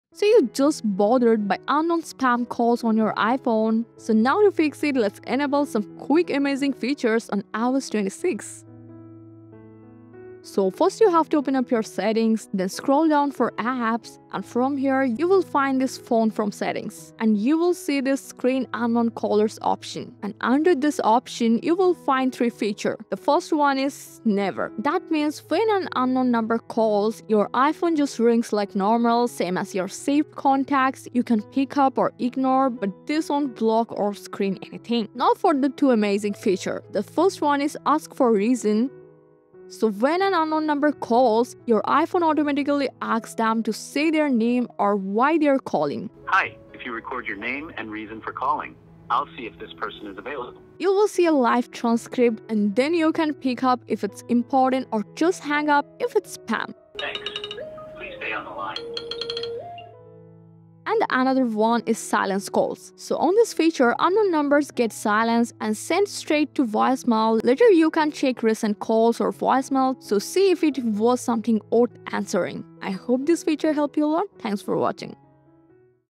iOS 26 - How To Use Call Screening on iPhone! [Enable]
After the iOS 26 update, you can use the new Call Screening feature to manage incoming unknown calls on any iPhone, including iPhone AIR, 17, 16, 15, 14, 13, 12, and 11 Pro Max?

Easily enable new call screening on any iPhone with iOS 26 when tired of spam calls, telemarketers, or unknown numbers interrupting your day, want to know how? Follow the step-by-step guide to Turn On the Call screen on your iPhone running on iOS 26 or later effectively.